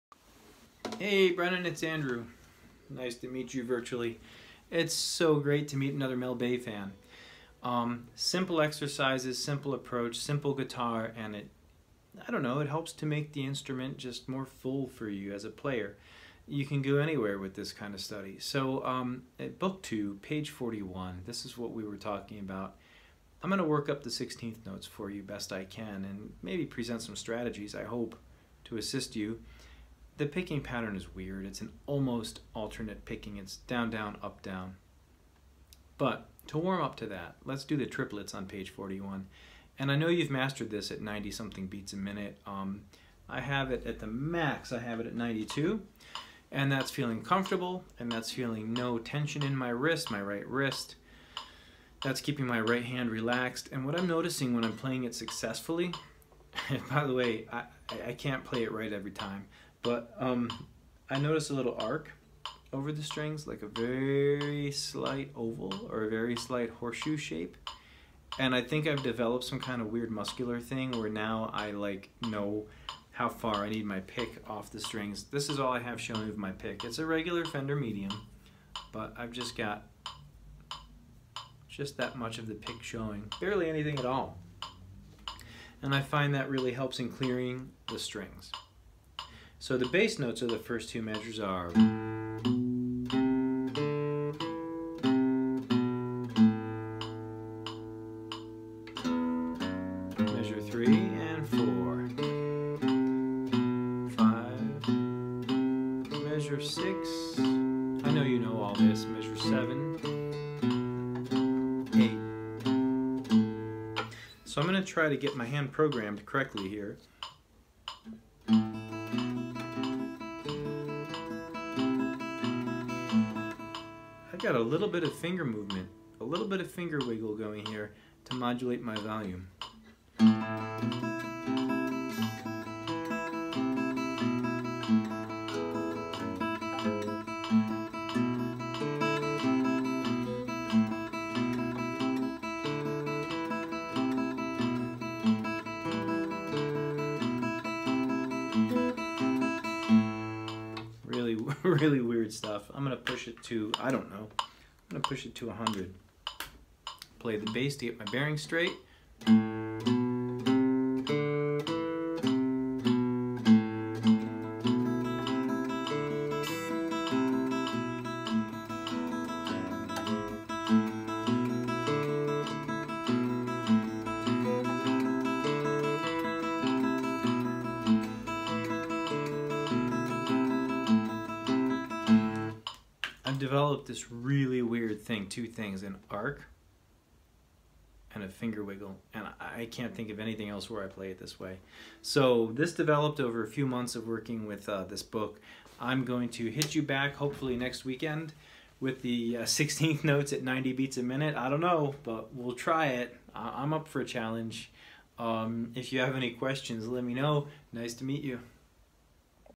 Mel Bay Modern Guitar Method Grade 2 Pick Plectrum Technique - Triplets - acoustic guitar technique
Shout out to Brennan! Will be posting sixteenth notes as well soon! EXERCISE PLAYED AT 2:10 in video